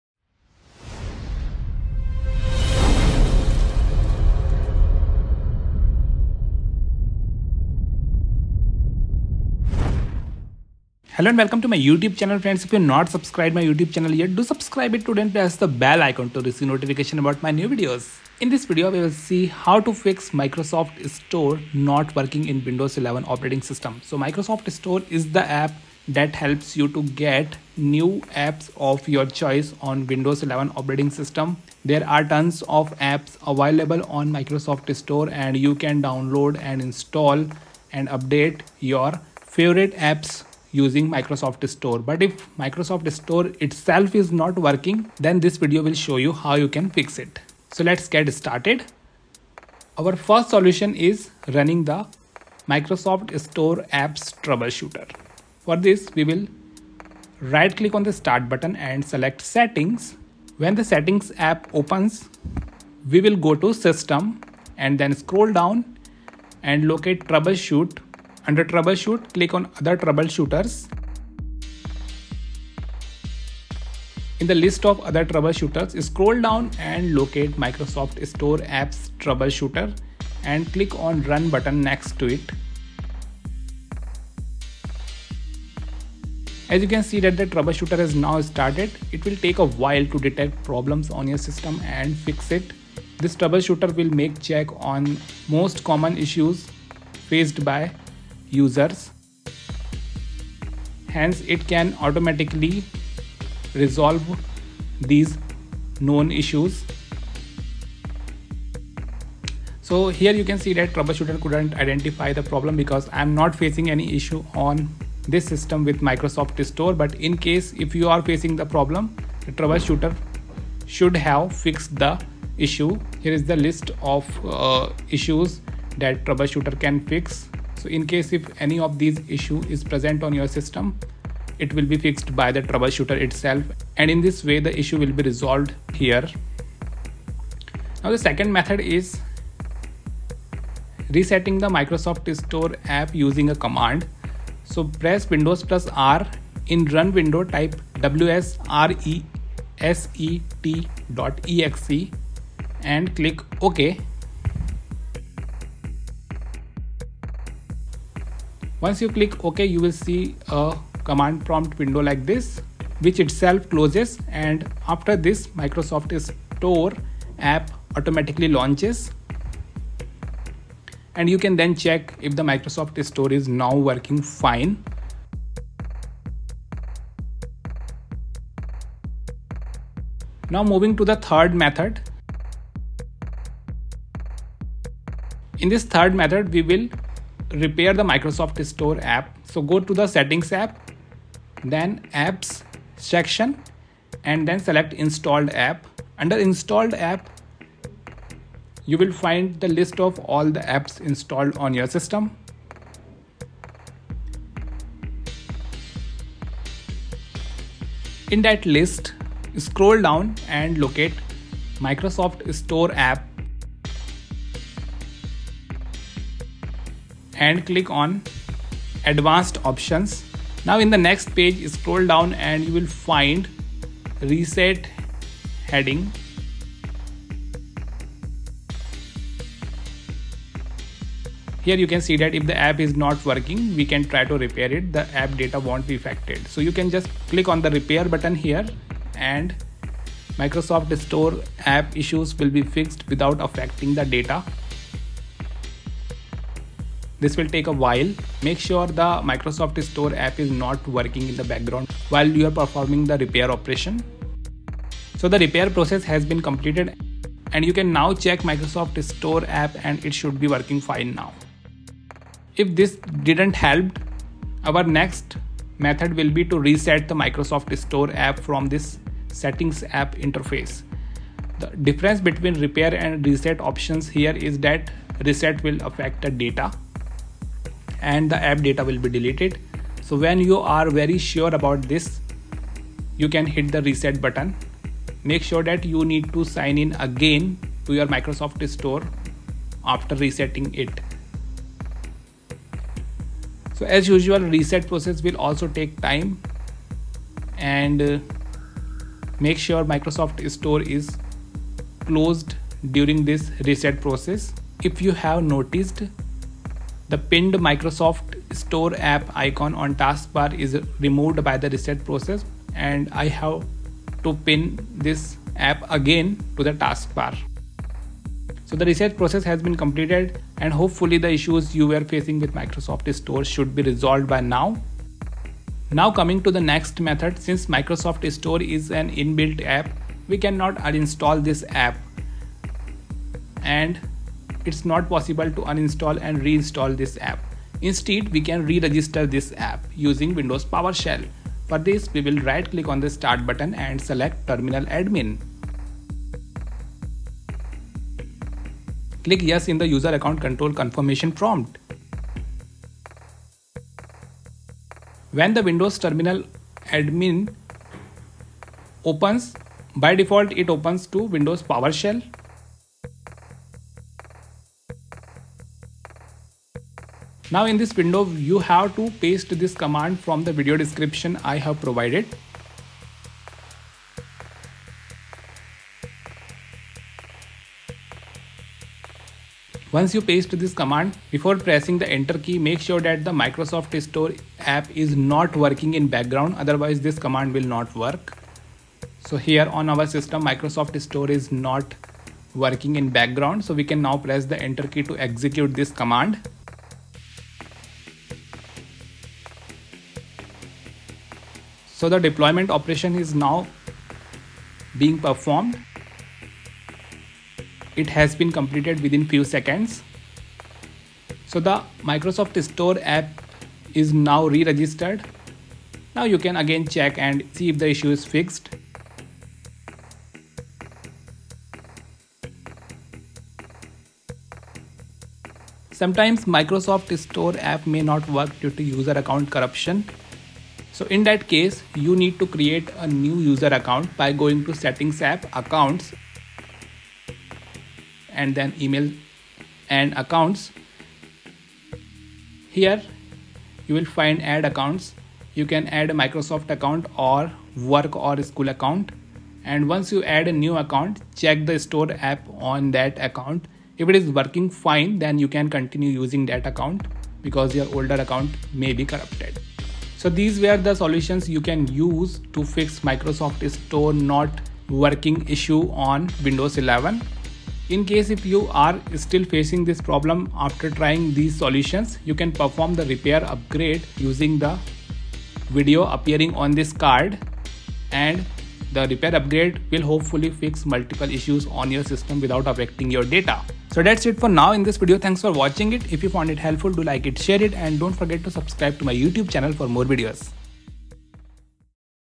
Solved: Microsoft Store not working in Windows 11
This video guide will show you how to fix if Microsoft Store not working in Windows 11.

Chapters:
00:00 Intro
00:49 Solution 1
02:20 Solution 2
03:03 Solution 3
04:23 Solution 4
05:26 Solution 5
07:05 Solution 6
08:05 End screen